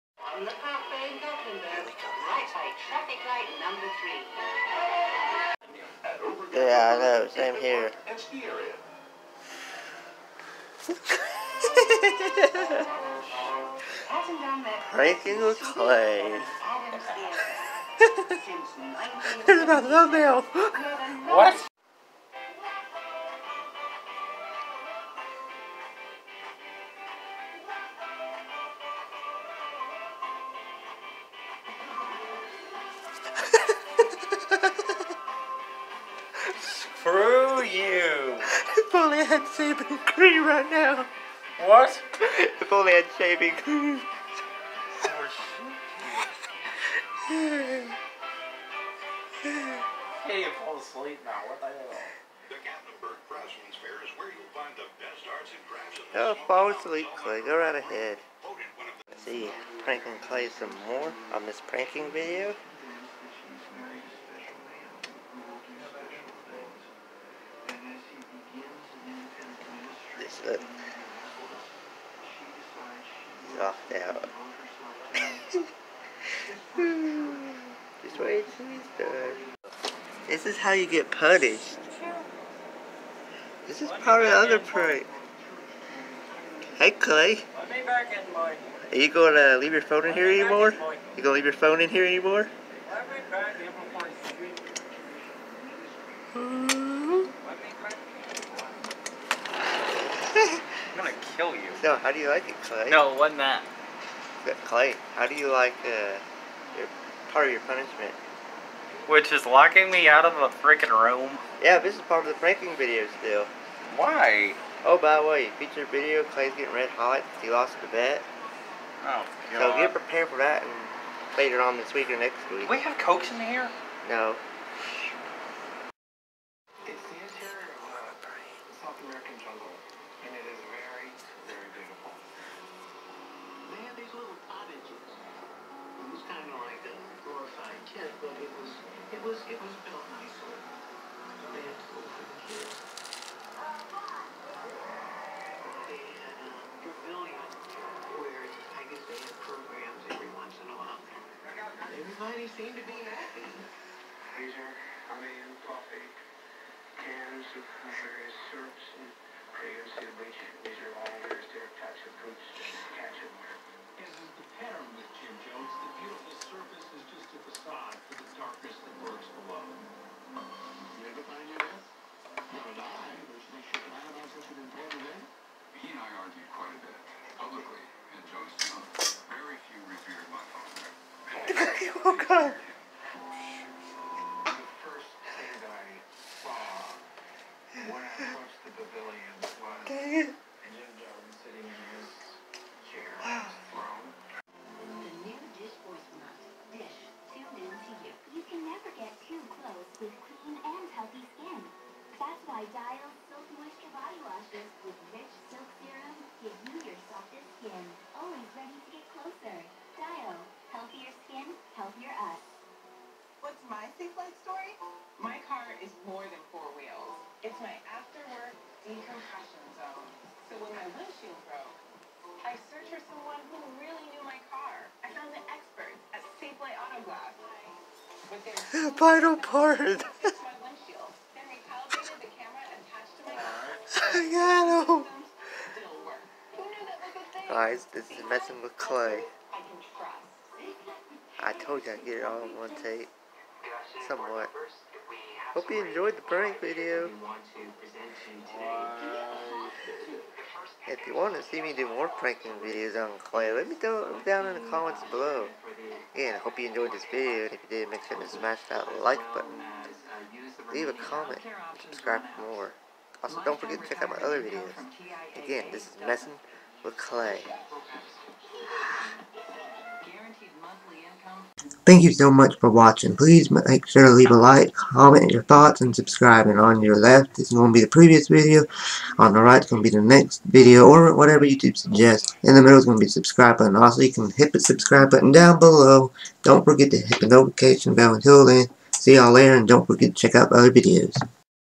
Pranking Clay in Gatlinburg hotel room
I decided to mess with Clay after he fell asleep. I know he was tired and so was I but I couldn't miss that opportunity. I know he thought it was funny. I also did it because he left his phone in the room which was goofy of him. Hope ya'll have a great day and don't forget to check my other videos out!!!

Hope Ya'll have a great day and see ya!!!